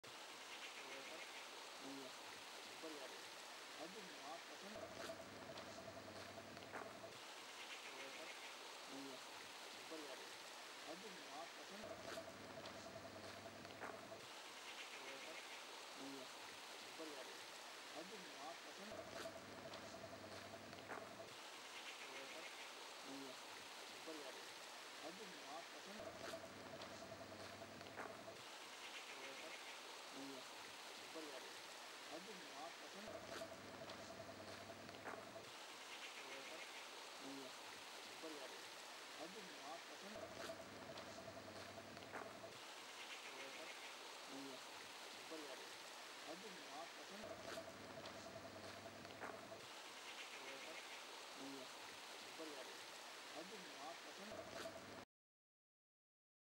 Big male Bluebull foraging in a forest
A large male Nilgai or Bluebull antelope foraging in a forest in India.